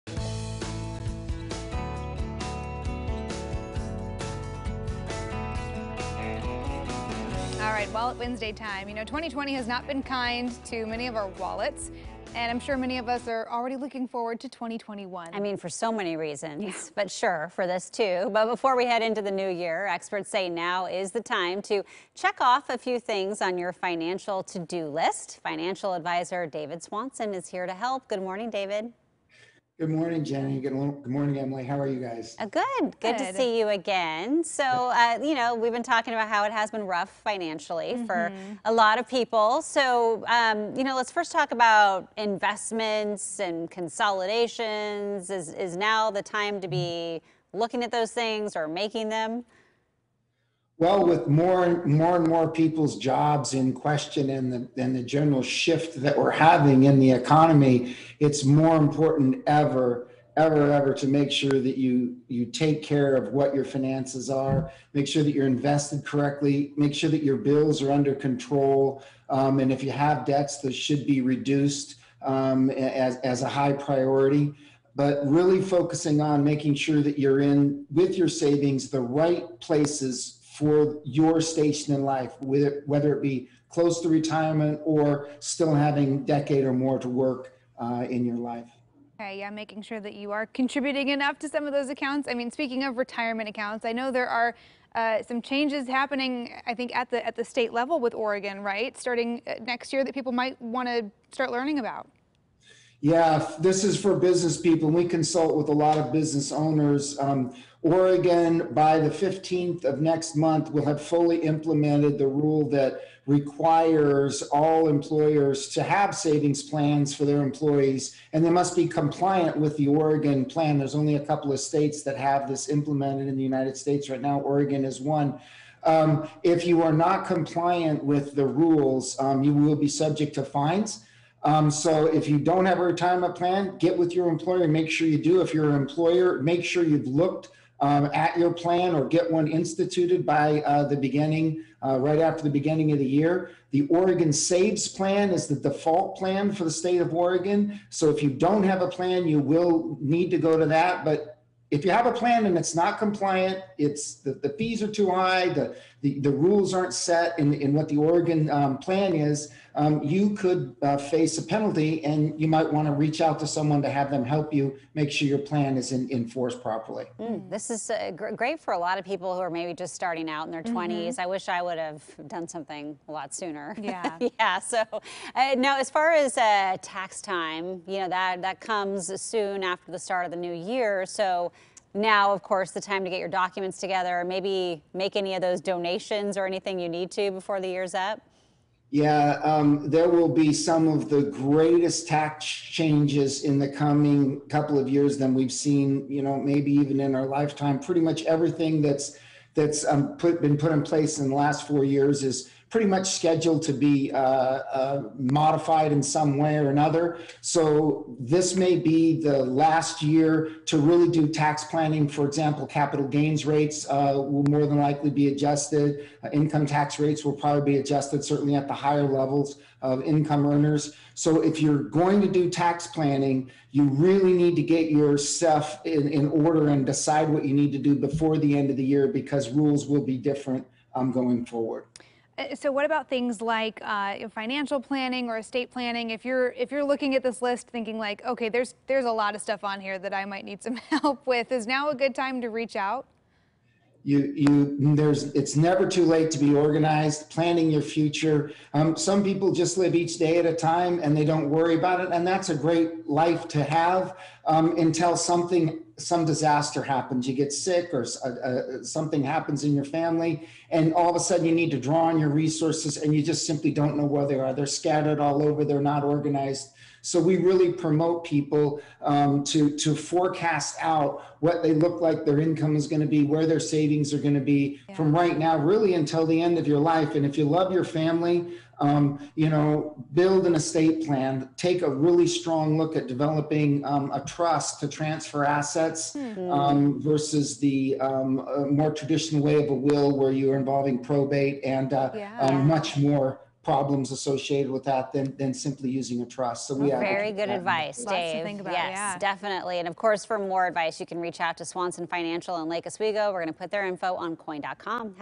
Wallet Wednesday: EOY Checklists
Before you head into the new year, experts say now is a good time to check off a few things n your financial to-do list.